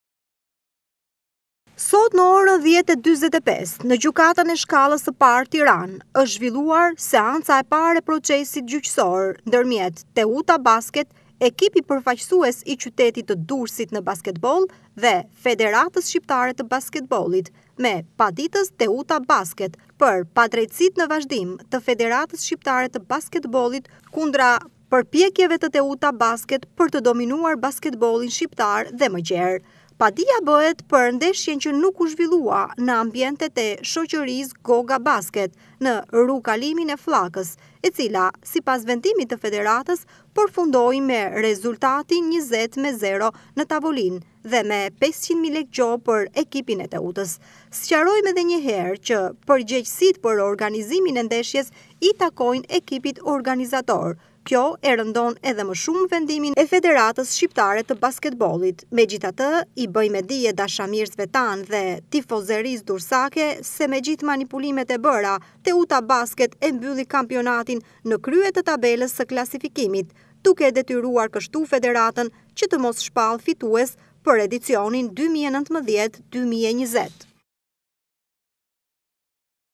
FSHB përballë "Teuta Basket", për padrejtësitë ndaj ekipit përfaqësues të qytetit bregdetar
Durres  Federata Shqiptare e Basketbollit përballë "Teuta Basket", për padrejtësitë ndaj ekipit përfaqësues të qytetit bregdetar